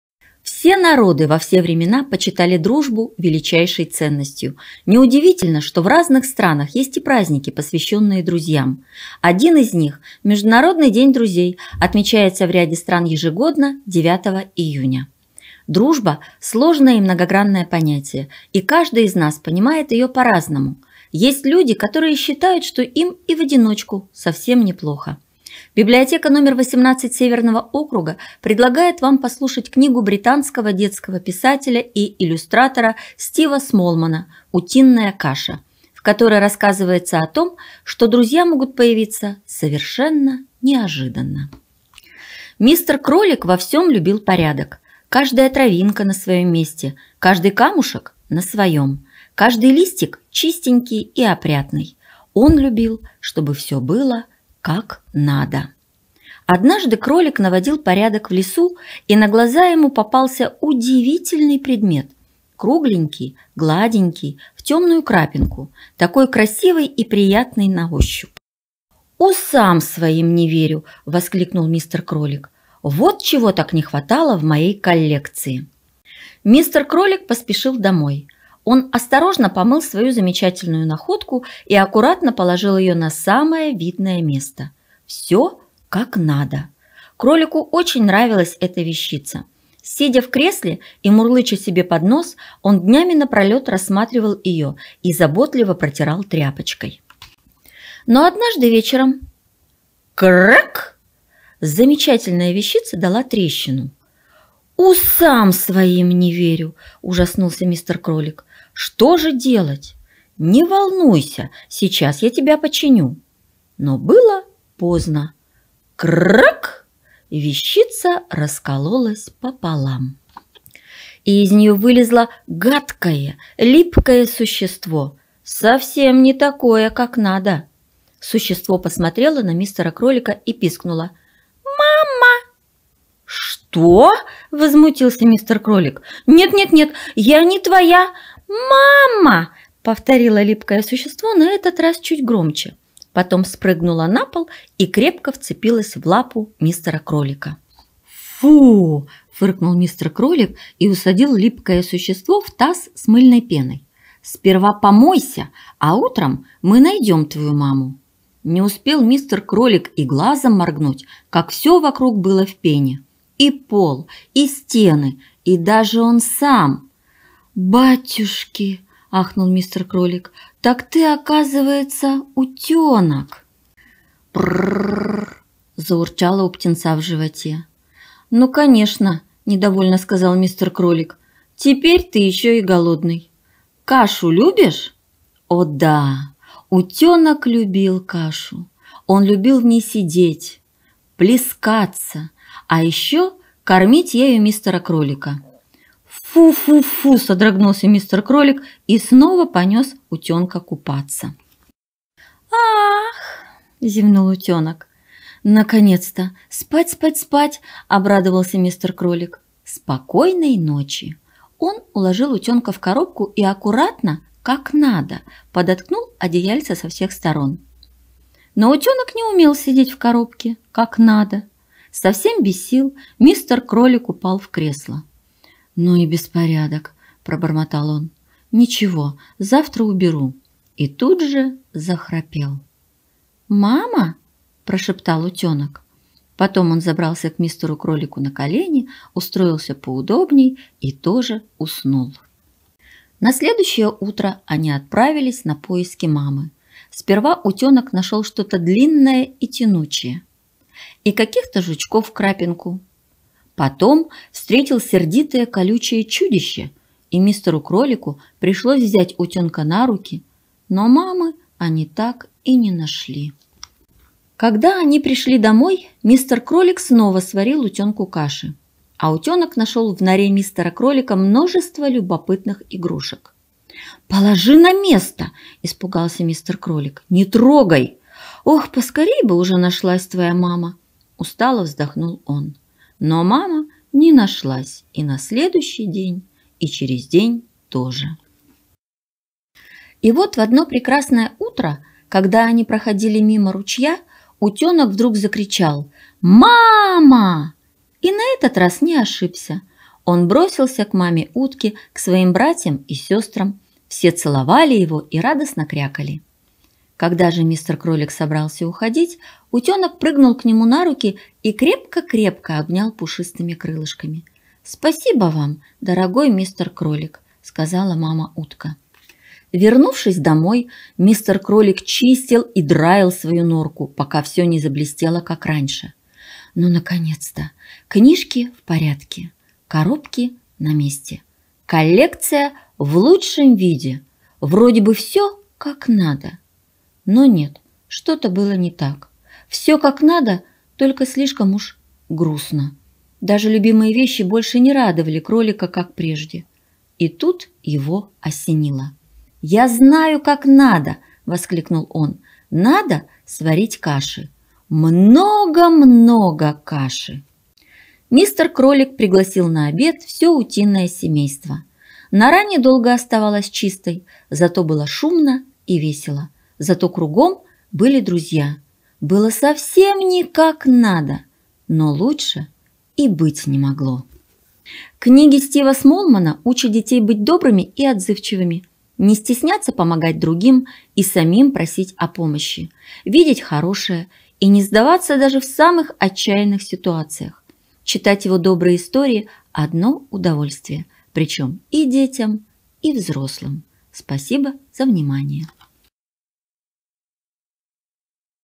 Стив Смолман «Утиная каша»
Читаем вместе с Библиотекой № 18 Северного округа